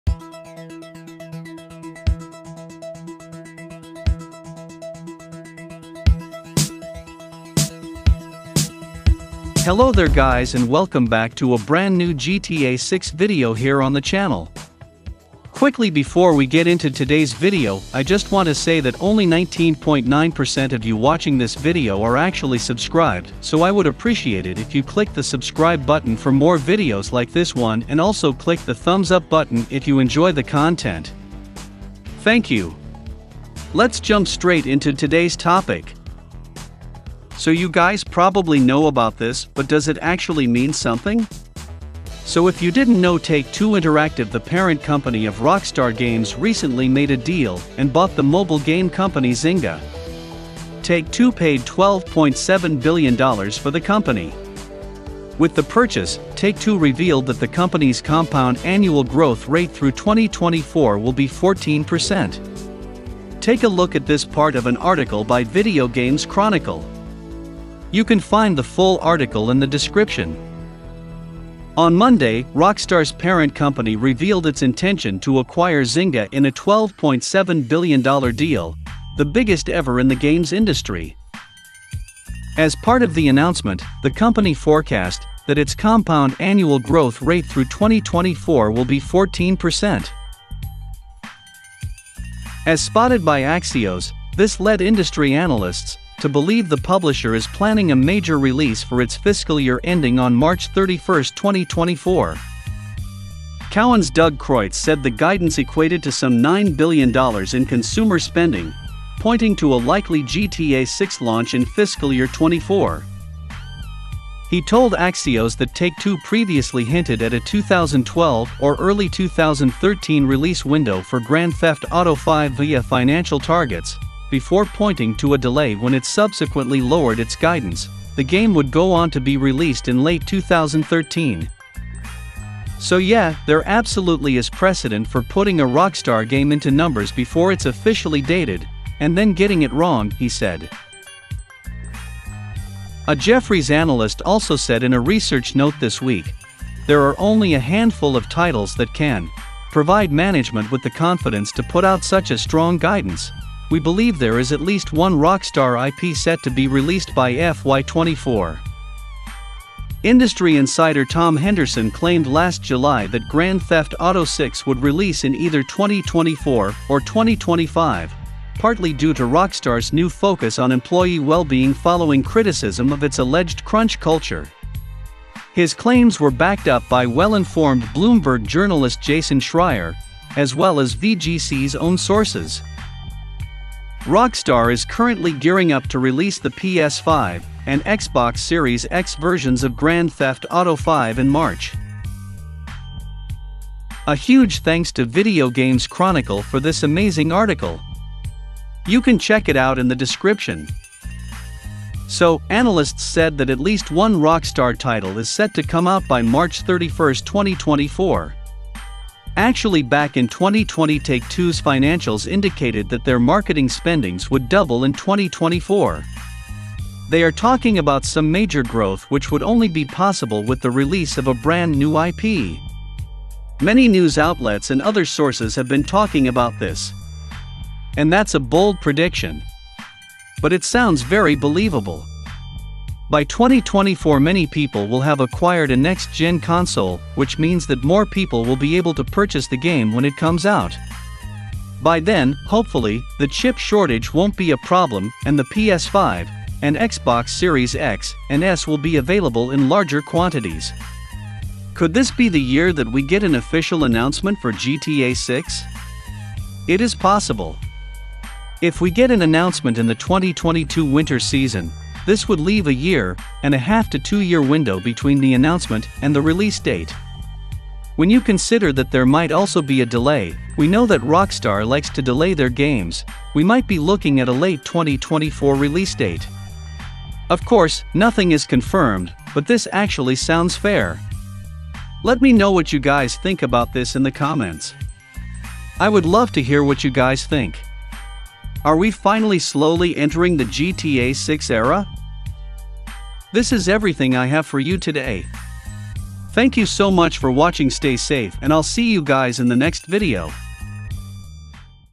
Take-Two Interactive Reveals GTA 6 RELEASE DATE?
With its recent report, Take-Two may have hinted at Grand Theft Auto 6’s planned release window! Analyst believes that the release date for Rockstar Games' Next Title May Have been officially REVEALED!
In today's Grnad Theft Auto 6 video we take a look at how Take-Two Interactive, Rockstar's parent company, hinted at the release date of Rockstar's Next Game!